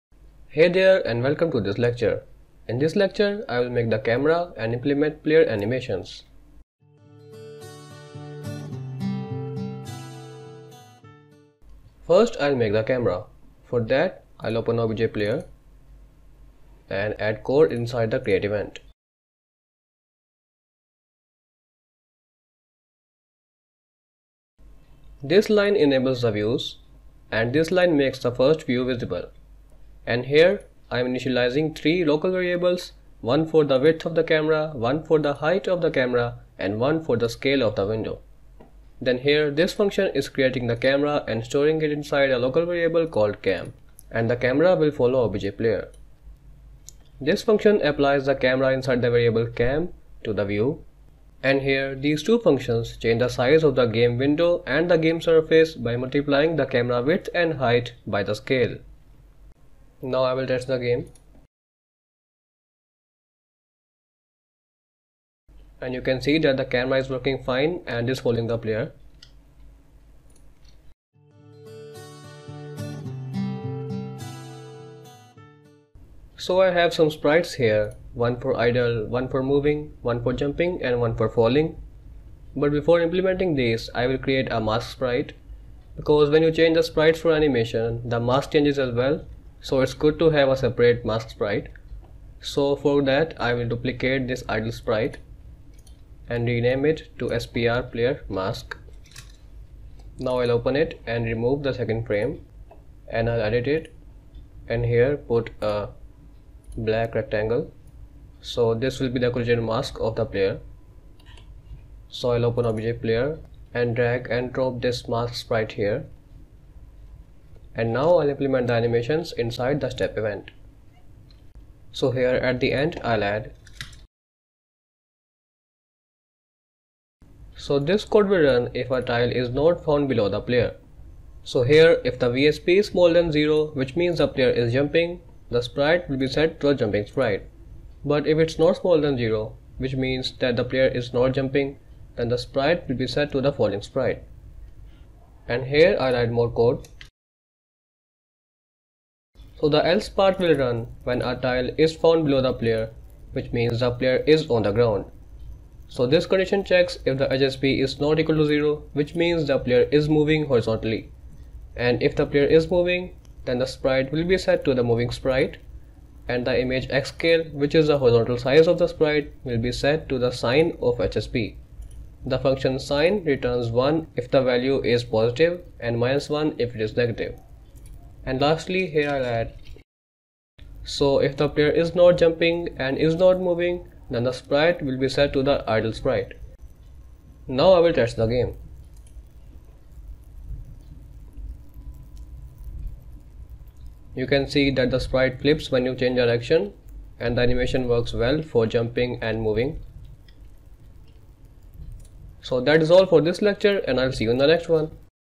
How to Create a Platformer with a Story - GameMaker Studio 2 : Camera & Animation
How to Create a Platformer with a Story - GameMaker Studio 2
Learn to build a platformer using GameMaker Language, while growing as a programmer!